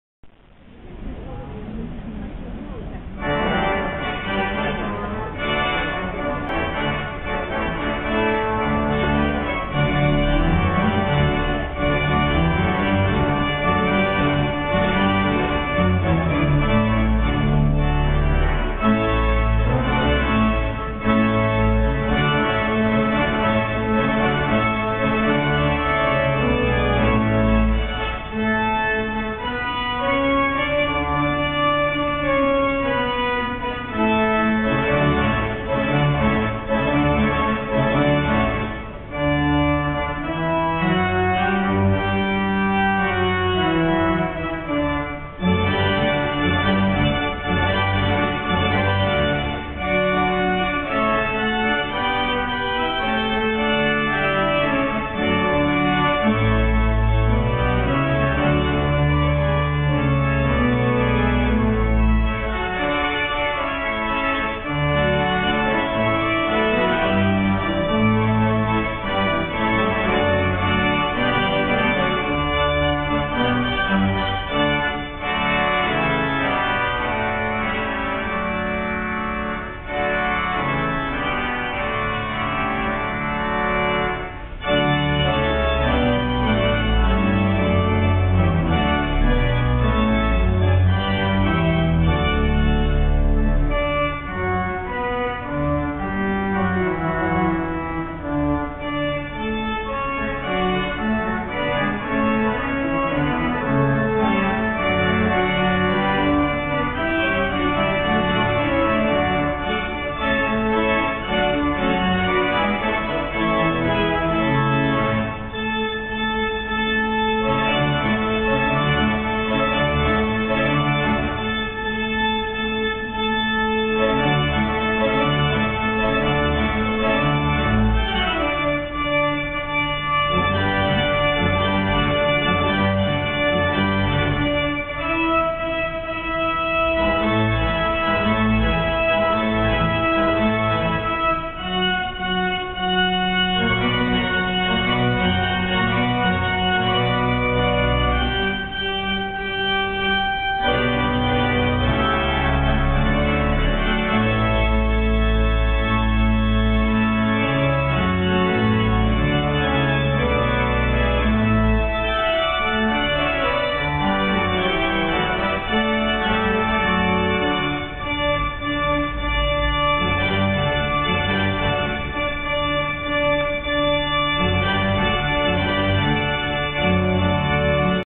Church Music 7 - Organ
Nov. 2010 Albert Street Uniting Church, - Free Lunchtime Concert by the Brisbane City Council